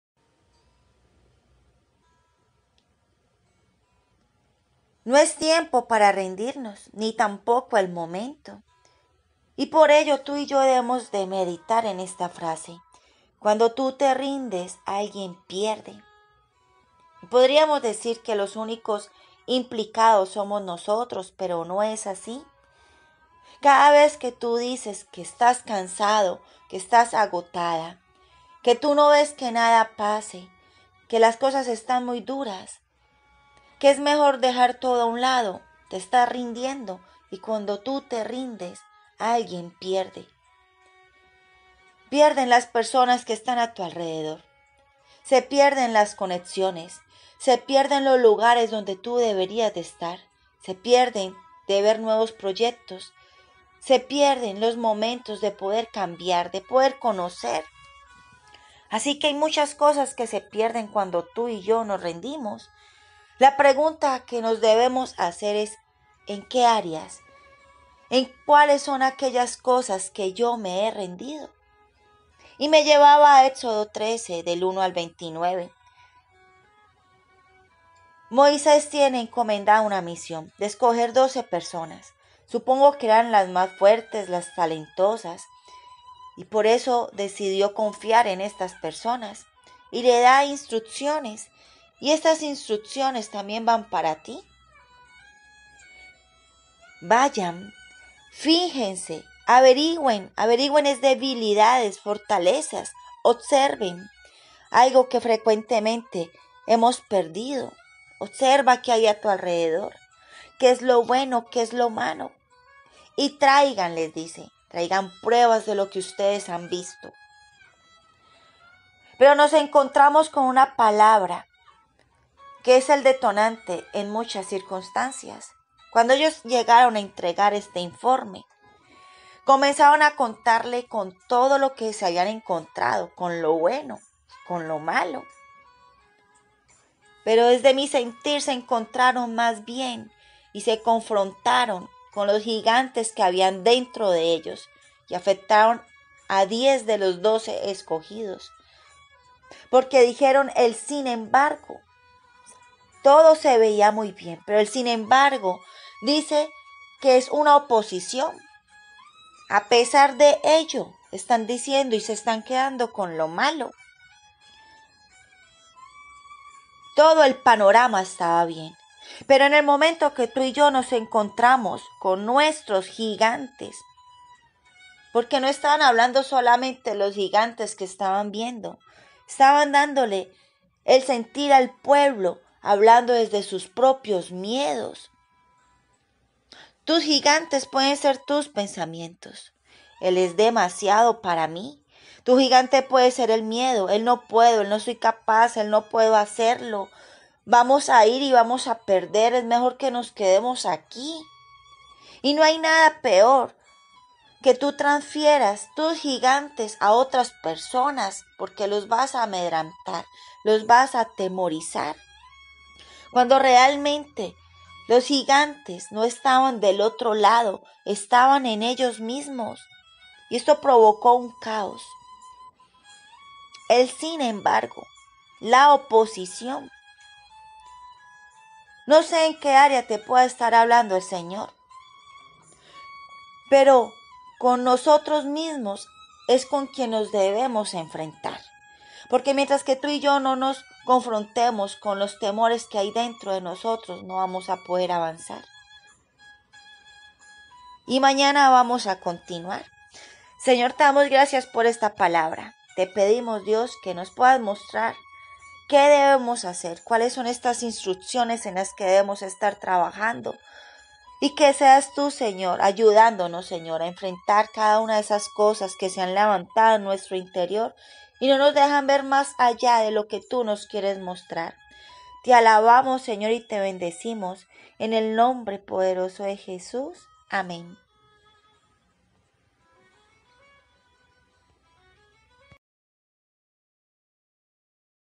CUANDO TE RINDES ALGUIEN PIERDE/ PALABRAS AL ALMA /MARIAE ALZATE
*Cuando tu te RINDES alguien PIERDE* la oposicion que hay desde tus propios miedos y temores en tu interior son lo que ganan cada vez que decides rendirte y no continuar.
El Señor está contigo, pero para avanzar debes dar un paso a la vez.
Éxodo 13:1-29
🥰🙏🏻Dios te bendiga siempre